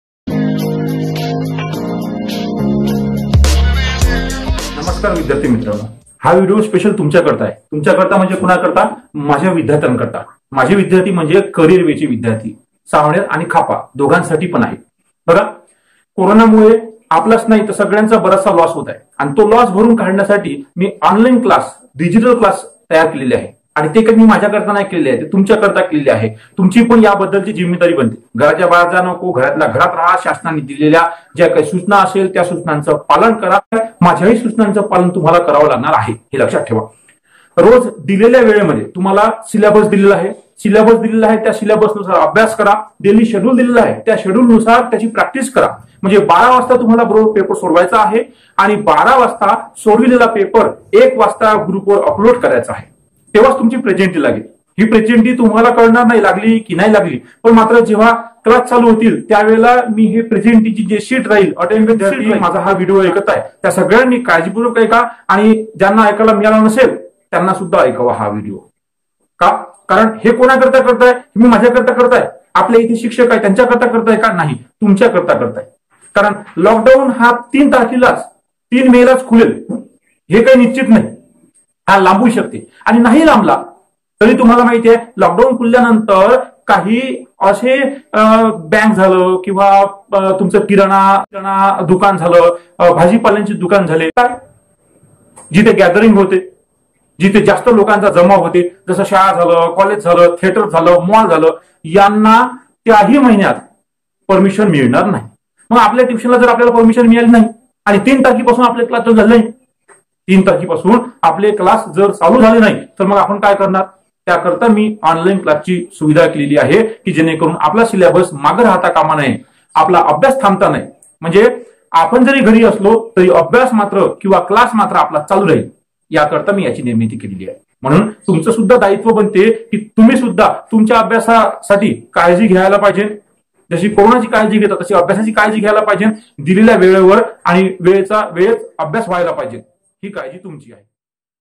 Director Speech for Our Academic Students During Covid - 19
Corona virus mude lockdown chi stiti nirman zaleli ahe ashya stitit academic study thambta kama naye mnun online class hot ahe hi mahiti samjaun sangatanna director Mr. Nitish Bhongade Sir